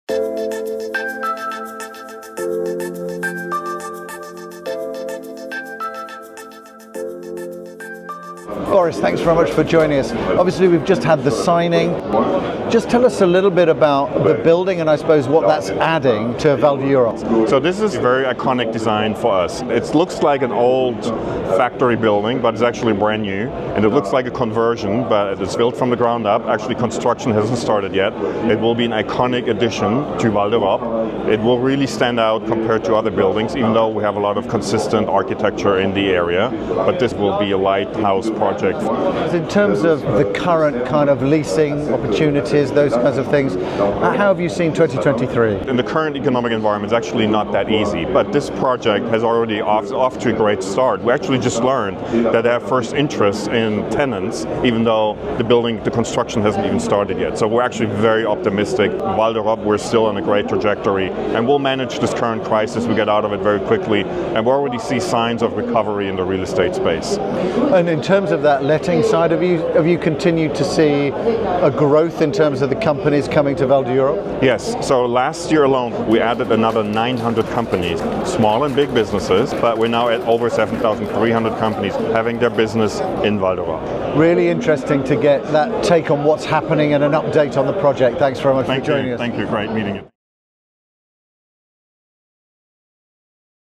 La Fabrique sets new standard for Val d'Europe: Boris Solbach, Disneyland Paris
During a signature ceremony held during last month’s MIPIM, senior vice-president business strategy and finance and chief financial officer of Disneyland Paris Boris Solbach described the proposed building, La Fabrique, part of the emerging Val d’Europe urban development as “a very iconic design".

Solbach explained that although the building will look like an old factory, it will be brand new and will be built from the ground up and its use will not be industrial.

“It will really stand out compared to other buildings," Solbach told Real Asset Insight’s Richard Betts. “This will be a lighthouse project for the area.”

Although he acknowledged that the current economic environment “is actually not that easy”, Solbach said the project, which will provide 2,915 sq m of offices and 1,400 sq m of retail space, is off to a great start and the first interest from potential tenants has already been registered, even before construction starts.

“We're actually very optimistic. Obviously the current economic environment makes it a little bit harder for investors so we have to be a little more careful on what we ask and how we position it.”

He said Val d’Europe is on a great trajectory. “We'll manage this current crisis, we will get out of it very quickly and we already see signs of recovery in the real estate space.”

He pointed to the growing number of companies locating at Val d’Europe.

“Last year alone we added another 900 companies, small and big businesses. There are now over 7,300 companies that have their business at Val d’Europe.”